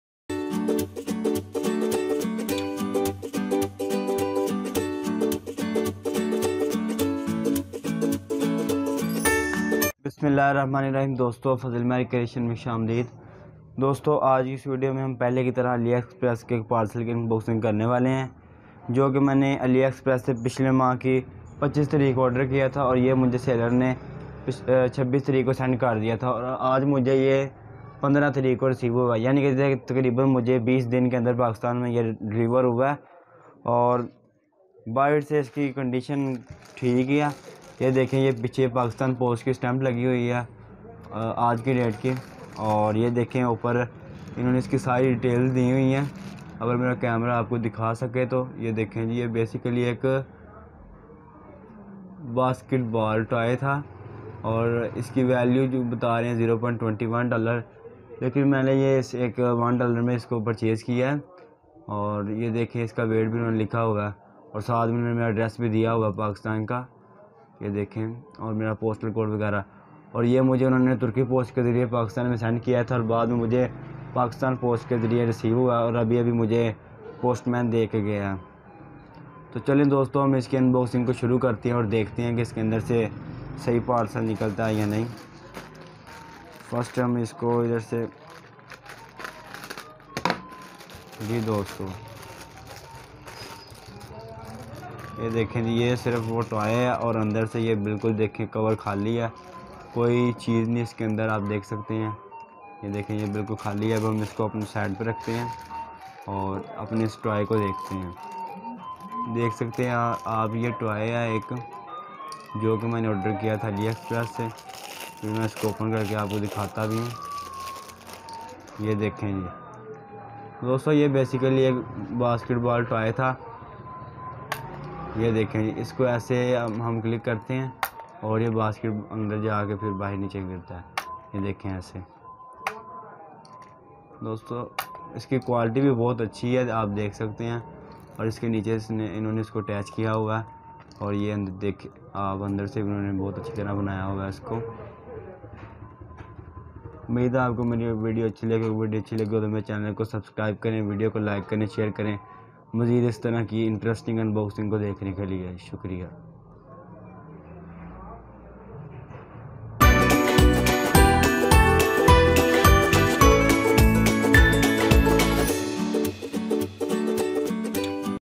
AliExpress Parcels Unboxing in Pakistan | Unboxing and Review | Fazal Maher Creation 18
Hi Friends ! In this video I will Unbox a AliExpress Parcel which deliver From China to Pakistan. In only 25 Days. Thanks Aliexpress Shopping Pakistan Free Product